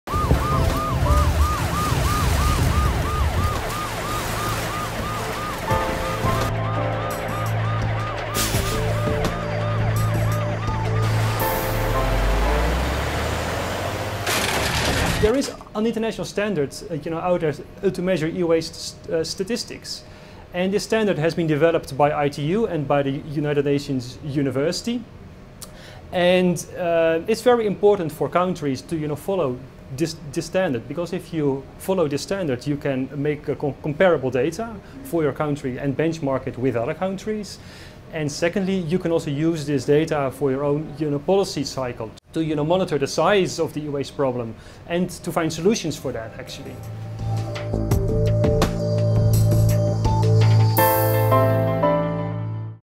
Why e waste standards are important?
Dr. Kees Baldé is a Senior Programme Officer at the Sustainable Cycles (SCYCLE) Programme co-hosted at the United Nations University (UNU) and United Nations Institute for Training and Research (UNITAR). As a part of his job, he gives policy advice to countries on e-waste. He stopped by ITU to answer most common questions on e-waste. Recorded in September 2019.